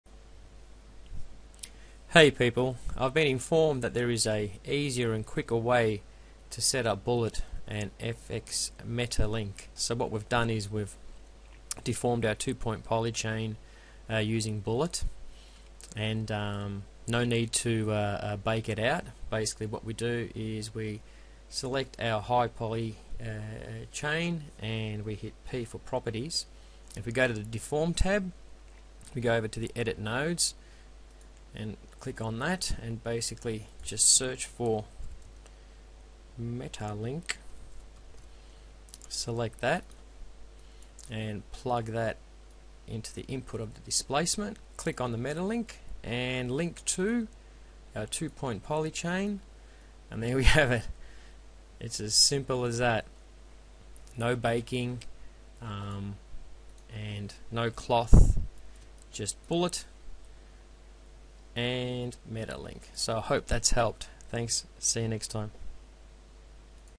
Bullet Dynamics & Metalink NODE
This is a much simpler way to use metalink, doing away with the mdd file and clothFX.
Simply a case of deforming the 2 point poly chain with Bullet and using the metalink NODE on the high poly object. Its great cos you can continue to edit the bullet dynamics with metalink still linking.
Thanks Lino!